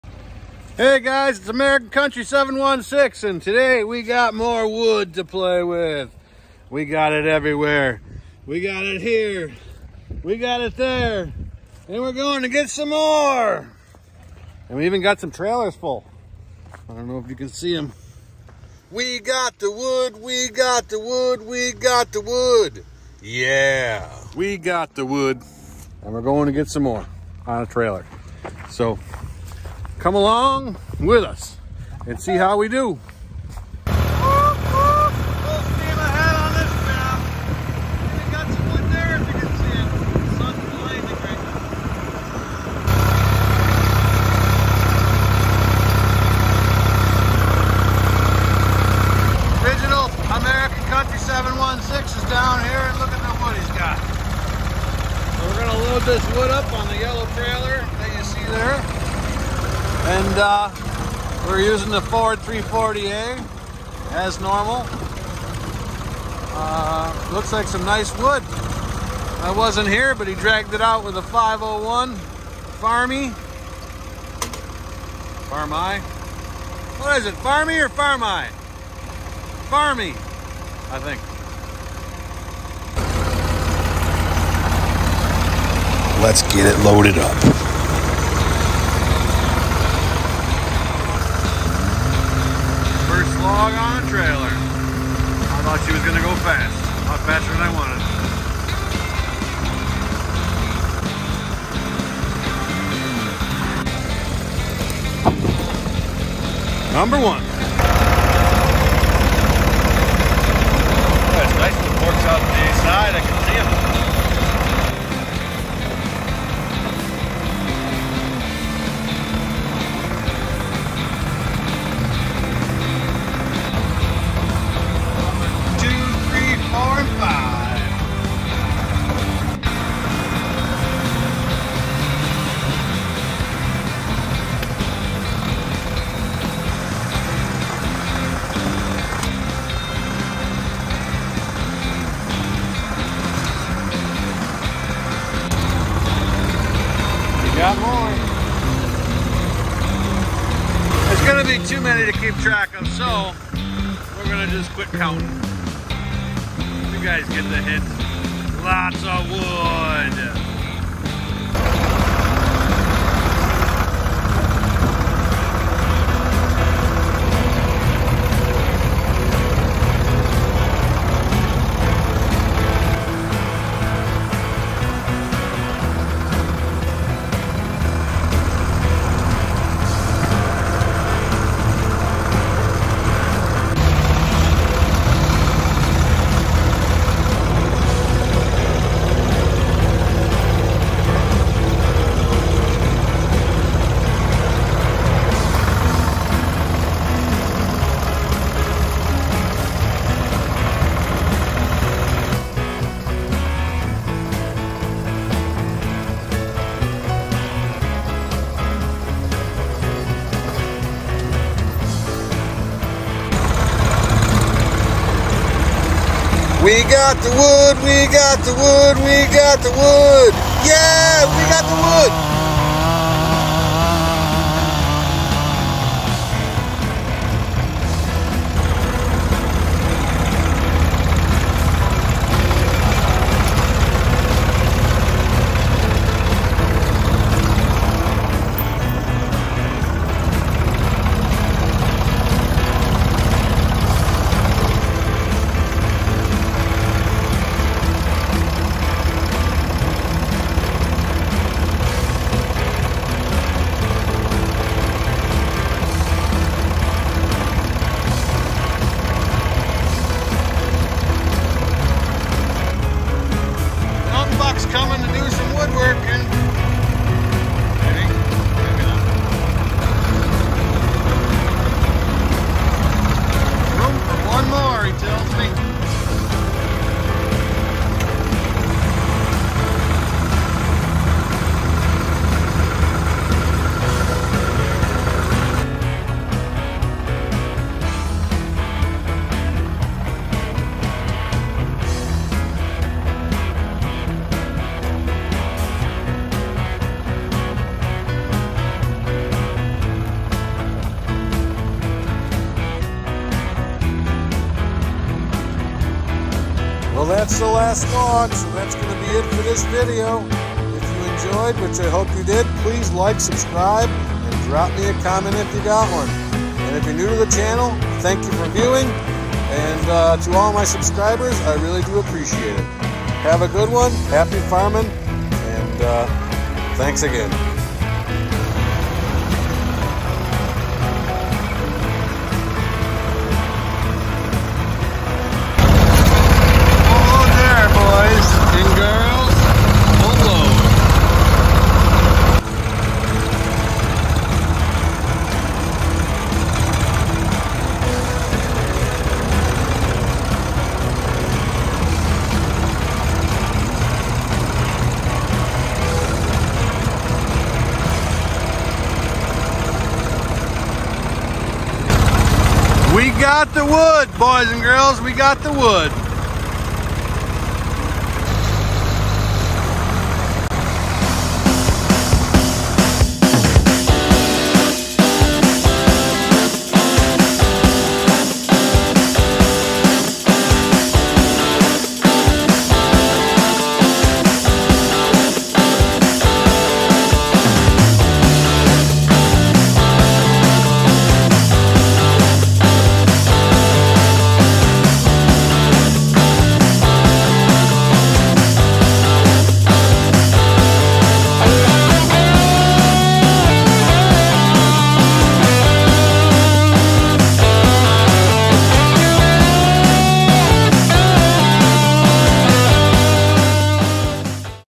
We Got The Wood
We are gathering wood so we can split it on our wolf Ridge 28 Pro. We are using our Ford 340A and Husqvarna 562 XP. Come along for this short journey. Firewood.